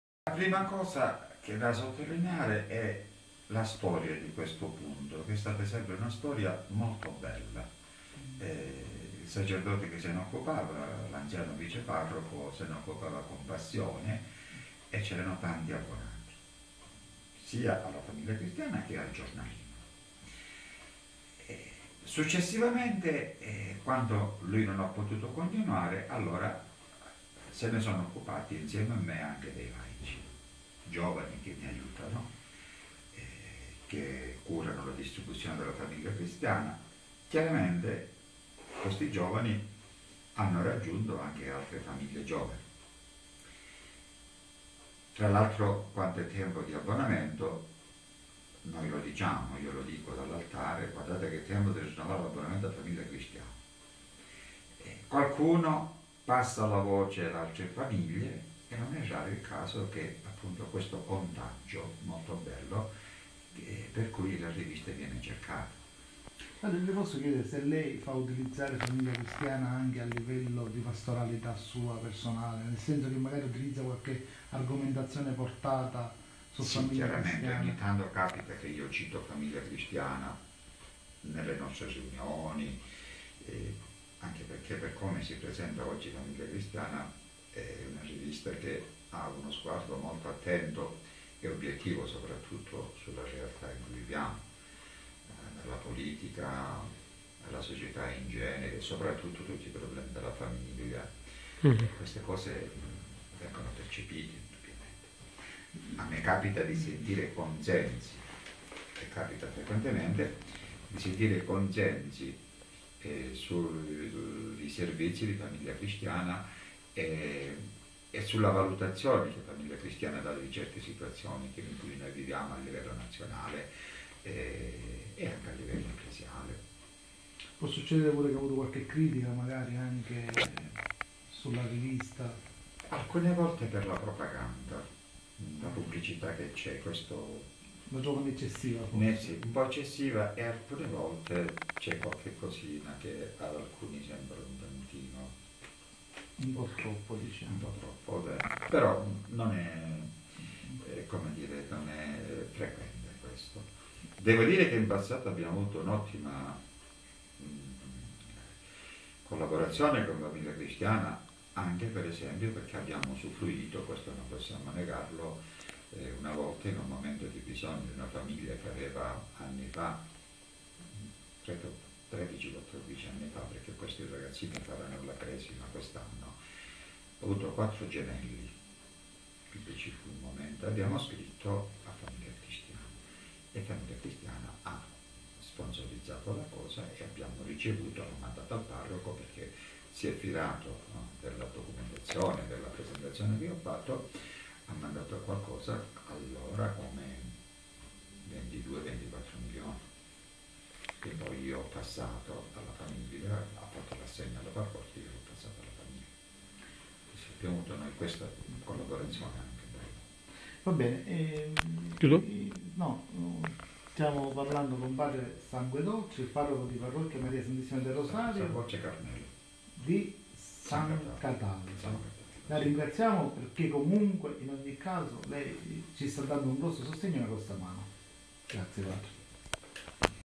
Nicola Di Piazza - intervista a P.Carmelo Sangudolce Diocesi di Caltanisetta
alcuni funzionari della Periodici San Paolo chiedono a Padre Sangudolce della Parrocchia M.SS. del Rosario di San Cataldo (CL) come mai nella sua parrocchia il Periodico dei Paolini , Famiglia Cristiana sia cosi letta.